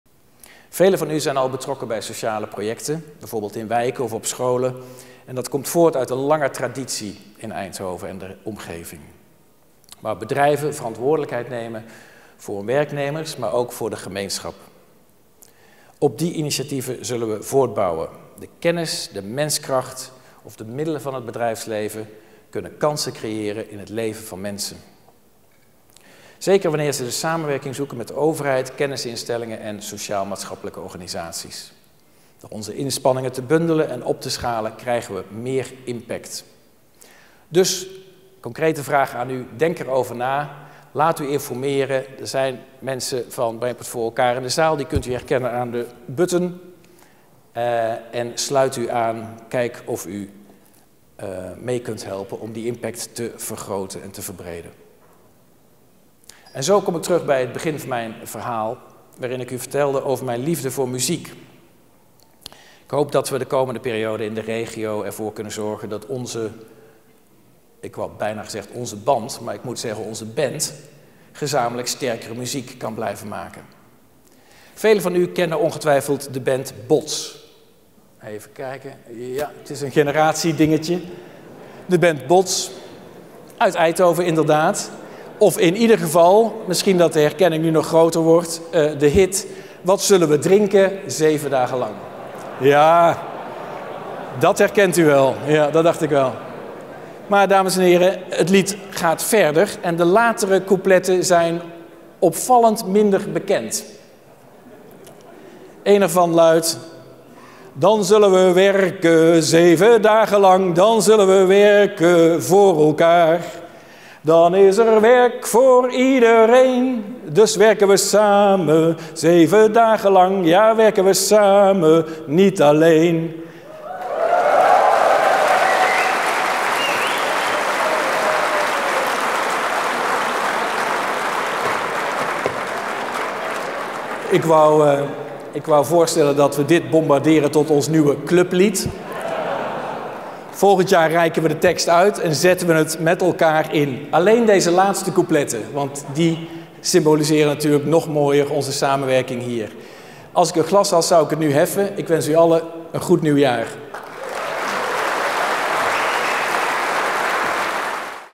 Speech Jeroen Dijsselbloem The New Horizon 2024 AFSLUITING
Het laatste gezongen gedeelte van de speech van voorzitter van Stichting Brainport; Jeroen Disselbloem. Lees ook de volledige speech